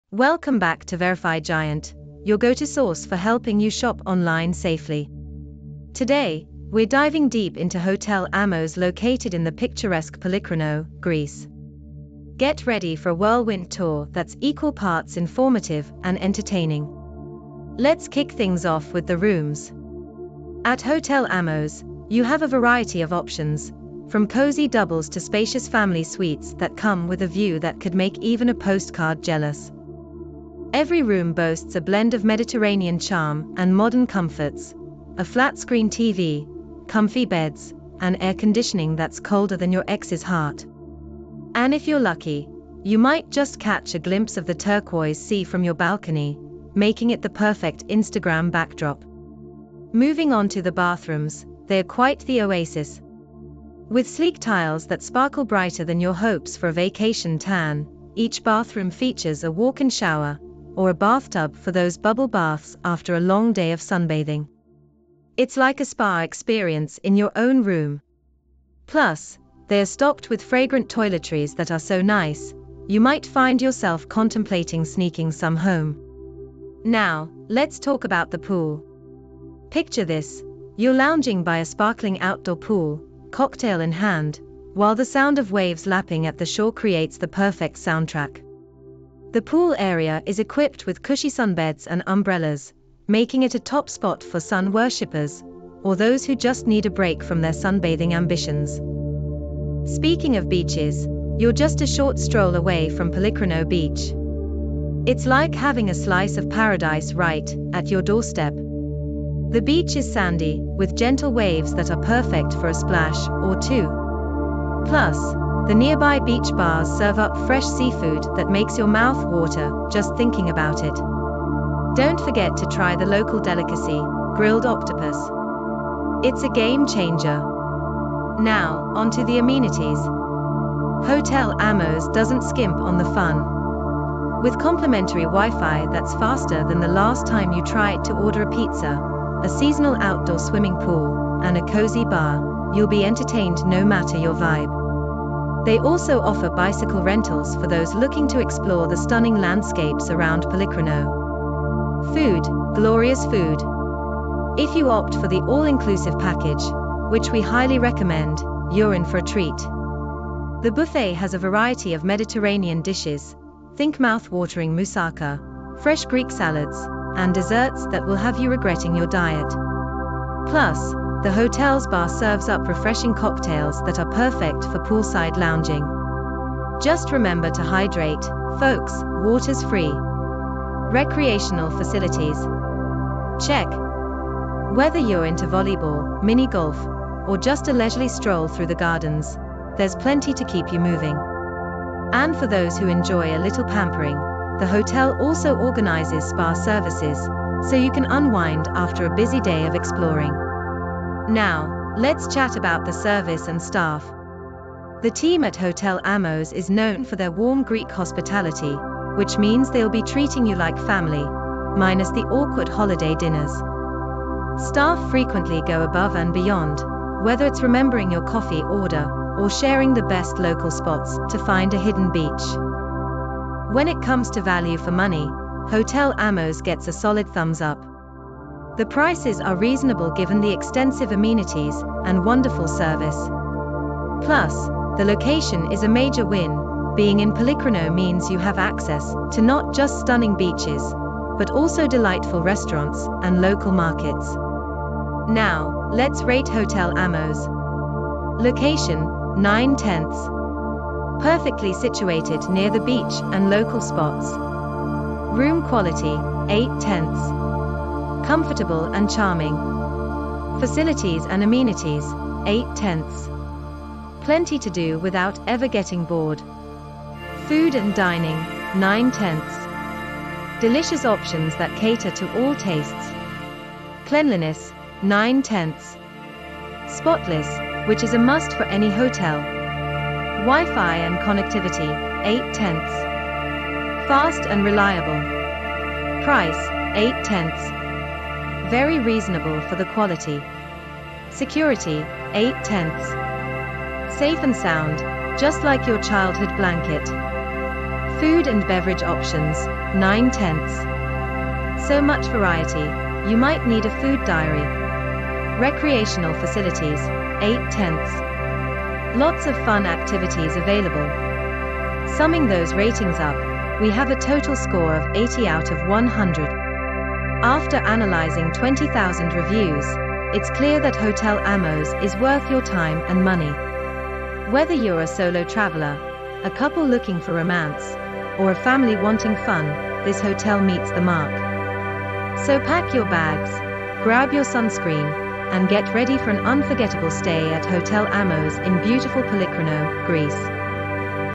Hotel Ammos, Polykhrono, Greece
Discover the beauty of Hotel Ammos, a stunning Mediterranean hotel nestled in the picturesque Polychrono, Greece! In this video, we take you on an exciting tour of this beach hotel, showcasing its cozy rooms, luxurious amenities, and warm Greek hospitality. From charming doubles to spacious family suites with breathtaking sea views, Hotel Ammos offers the perfect getaway for solo travelers, couples, and families alike.

Join us as we explore the oasis-like bathrooms, the sparkling outdoor pool ideal for sun worshippers, and the nearby Polychrono Beach, where sand and surf await just a short stroll away. Don’t miss out on the delightful dining options, including an all-inclusive package featuring Mediterranean cuisine that will make your taste buds dance!

With plenty of recreational facilities, bicycle rentals, and a cozy bar to sip refreshing cocktails, you’ll never run out of things to do at this top-rated hotel in Chalkidiki. Plus, we’ll give you insights into the affordability of this luxury stay in Greece and why it deserves its rave reviews.

Whether you're planning a vacation in Greece or looking for the best beachside hotels, our comprehensive review details everything you need to know about Hotel Ammos. Get ready to unwind, relax, and make unforgettable memories in the heart of Polychrono!